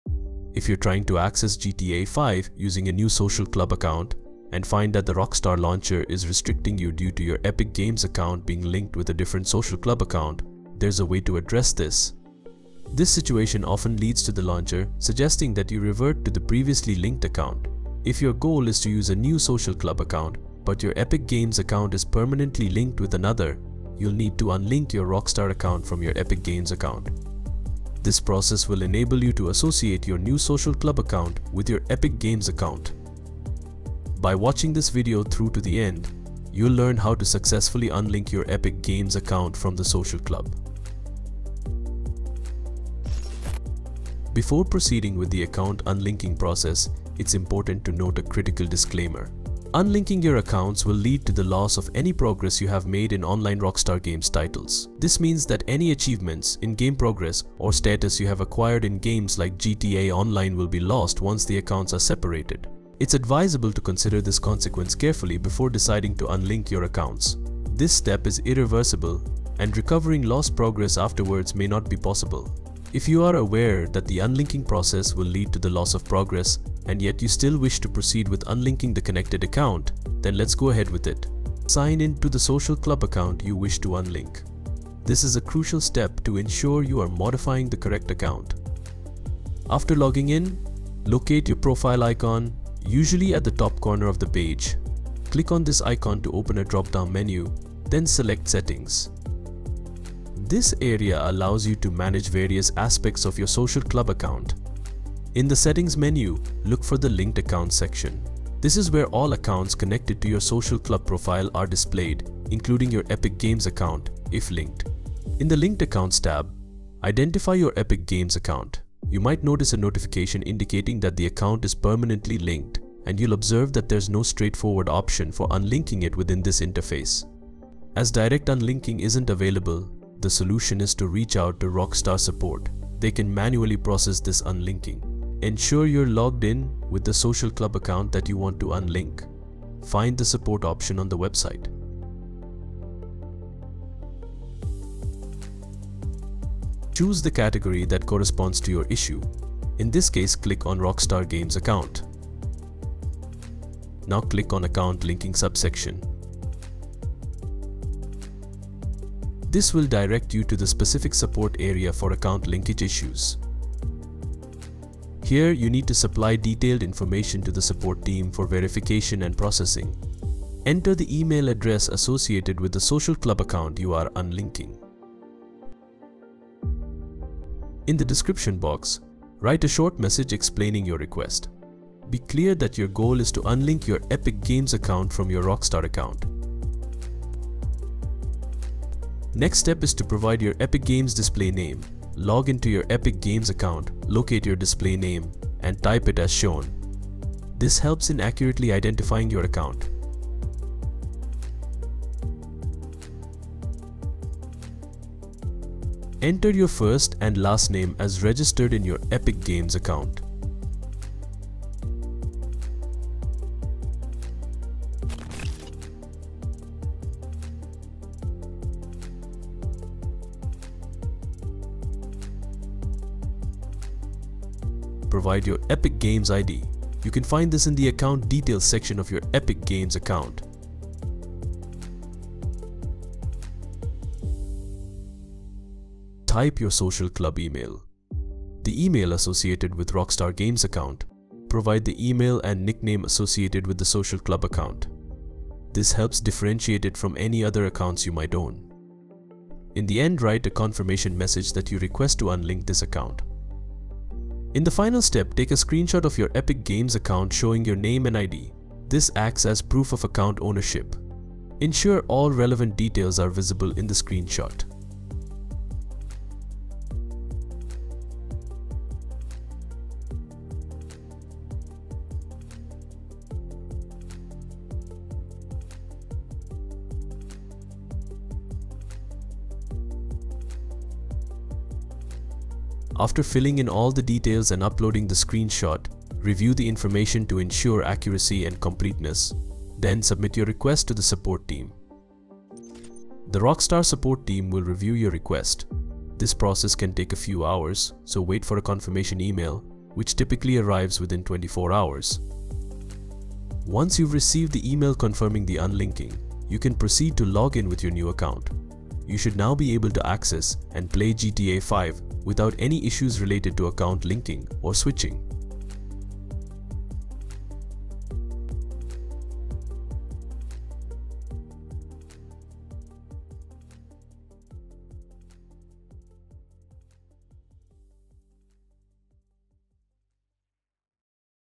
How to unlink Epic Games from Rockstar Account
Are you facing difficulties accessing GTA 5 with a new Social Club account because your Rockstar Launcher is linked to a different Social Club account through Epic Games? This video tutorial provides the perfect solution! We’ll walk you through the process of "Unlinking your Epic Games account from your Rockstar Games account," ensuring that you can associate your new Social Club account with Epic Games seamlessly.

Unlinking your accounts will result in the loss of all progress made in online Rockstar Games titles. This includes achievements, in-game progress, and status in games like GTA Online. Remember, this step is irreversible, and it might be impossible to recover any lost progress.

✅ What You'll Learn:

Detailed steps to "Unlink Rockstar Account From Epic Games."
Understanding the implications of unlinking your accounts.
Tips for a smooth transition to your new Social Club account.
💡 Follow along as we guide you through each step of "How to unlink Epic Games from Rockstar Account?" The process is straightforward, but requires careful attention to ensure that you are fully prepared for the changes that will occur.

By the end of this video, you'll have all the information you need to make an informed decision and complete the process successfully. "How Do I Unlink Platforms From My Epic Games" will no longer be a question you'll need to ask!

Remember, your gaming experience matters, and we're here to help enhance it. Let's get started!